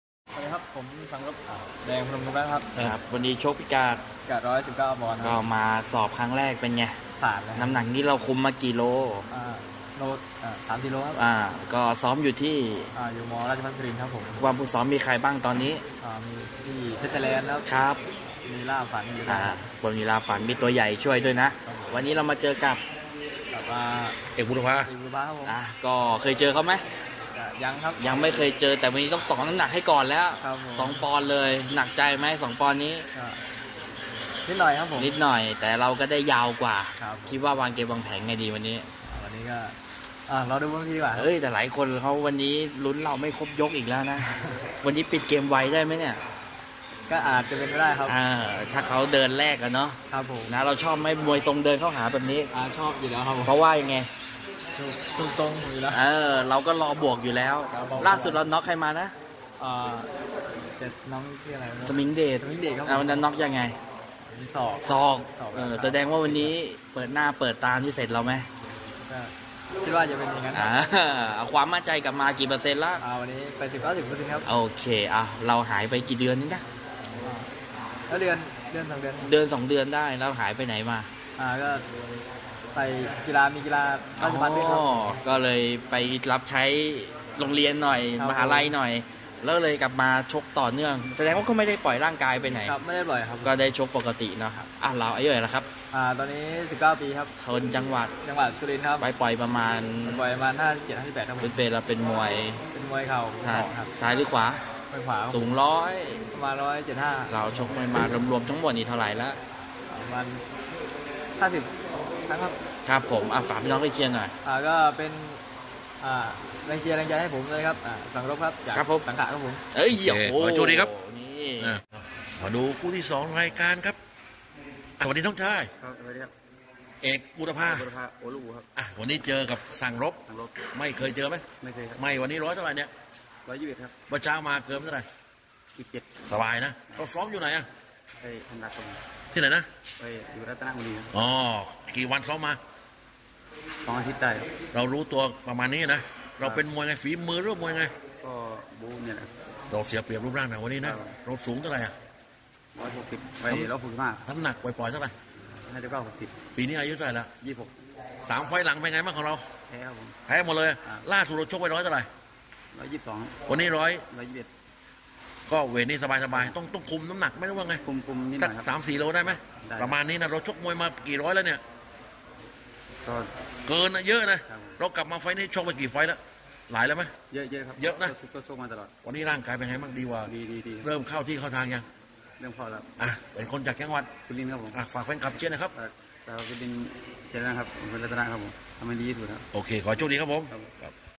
สัมภาษณ์ สั่งรบ VS เอกบูรพา ศึกมวยดีวิถีไทย ช่อง PPTV36 เริ่มชก 12.15 น. เวทีมวย อตก.3 จ.นนทบุรี
สัมภาษณ์ ชั่งก่อนชก มวยไทยวันนี้ สัมภาษณ์ สั่งรบ VS เอกบูรพา ศึกมวยดีวิถีไทย ช่อง PPTV36 เริ่มชก 12.15 น. เวทีมวย อตก.3 จ.นนทบุรี (21-01-67)